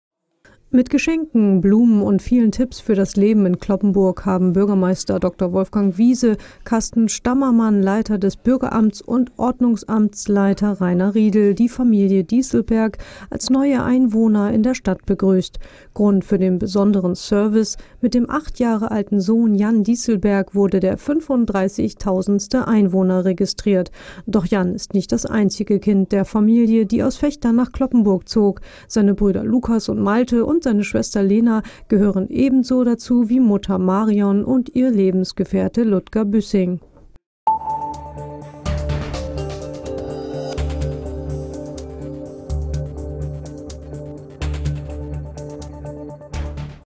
Jan Dießelberg ist Cloppenburgs 35000. Einwohner
Cloppenburg, 10. November 2015: Mit Geschenken und Blumen wurde die Familie Dießelberg als neue Einwohner in der Stadt  begrüßt. Grund für den besonderen Service: Mit dem acht Jahre alten Sohn Jan wurde der 35?000 Einwohner registriert.